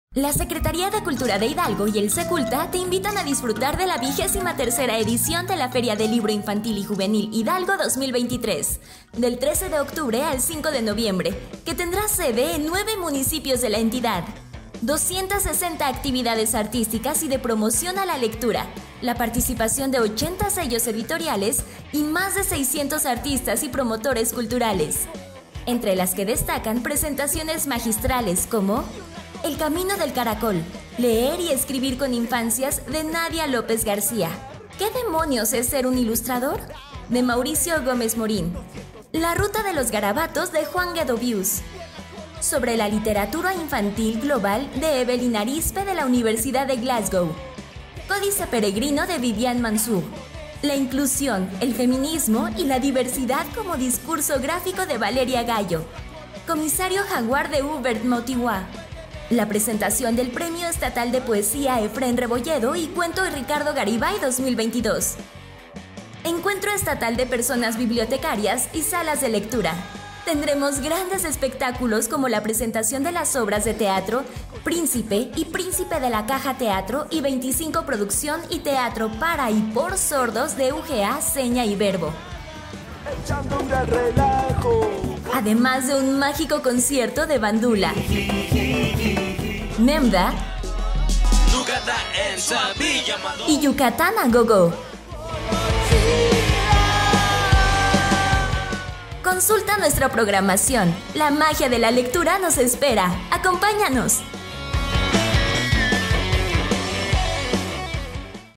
Promo FLIJH 2023
Del 13 de octubre al 5 de noviembre no te pierdas las actividades de la 23a Feria del Libro Infantil y Juvenil Hidalgo 2023 en el Centro Cultural del Ferrocarril.

La vocación central de esta edición es la de atender los derechos culturales de infancias, adolescencias y juventudes como un importante espacio de promoción de la lectura y el fomento al libro. 📖📚

📖Participarán más de 600 artistas y promotores culturales en 260 actividades, además de la presencia de 40 casas editoriales, por lo que esperan la asistencia de más de 15 mil personas.